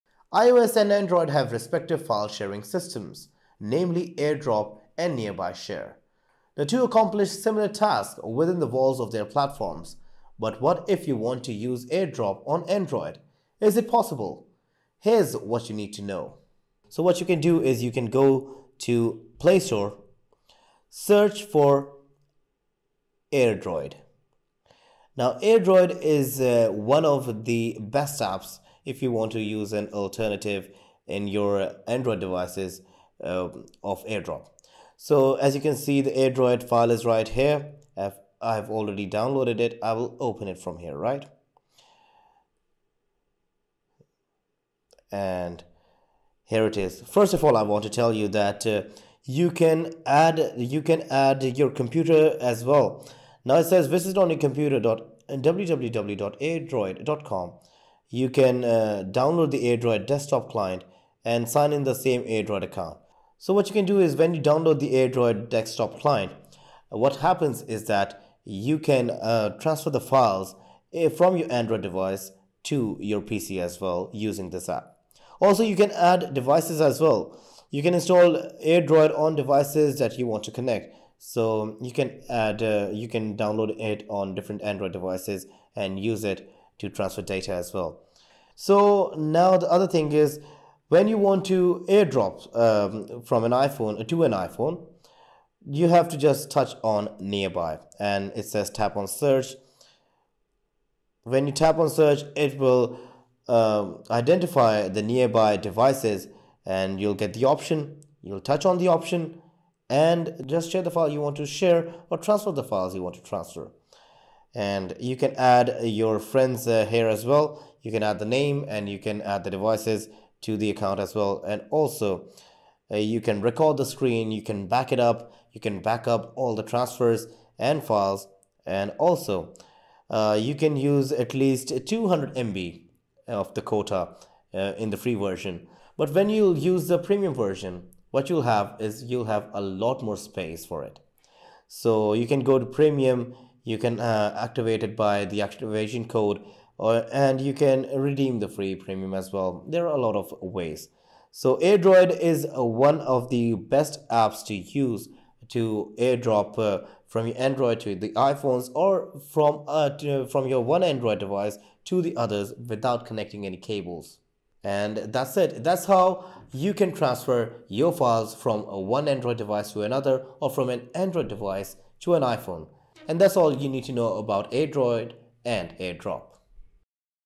How to AirDrop on Android (Using AirDroid app)
📱🔄 Airdrop on Android: How to Transfer Files between Android Devices and iPhones! 🔄📱

iOS and Android each have their file-sharing systems: Airdrop for iOS and Nearby Share for Android. While they serve similar functions within their platforms, you might wonder if it's possible to use Airdrop on Android. In this video, we'll explore how to achieve seamless file transfers between Android devices and iPhones using AirDroid.

📌 JUMP TO KEY SECTIONS:
0:00 - Introduction
0:17 - Download AirDroid app
0:46 - Adding Devices on Android
1:32 - Sharing Files to iPhone Devices
2:09 - Premium Features and Benefits

📲 How to download and use the AirDroid app:
1️⃣ Visit the Play Store and download AirDroid.
2️⃣ Open AirDroid, sign in with your account, and add your devices.
➡️ To add your devices for computer access, to download the AirDroid desktop client. For mobile devices, install the AirDroid app on your device.
➡️ Sign in to the same account. Once set up, you can easily transfer files between your devices.

📱 Sharing Files from iPhone to iPhone:
1️⃣ Tap "Nearby Share."
2️⃣ Tap "Tap to Search" to find nearby devices.
3️⃣ Select the desired device and share the file.

💡 Premium Features and Benefits:
➡️ Enjoy additional file transfer capacity beyond the free version.

🔗 For more information and tips on file sharing between Android and iOS devices, visit our article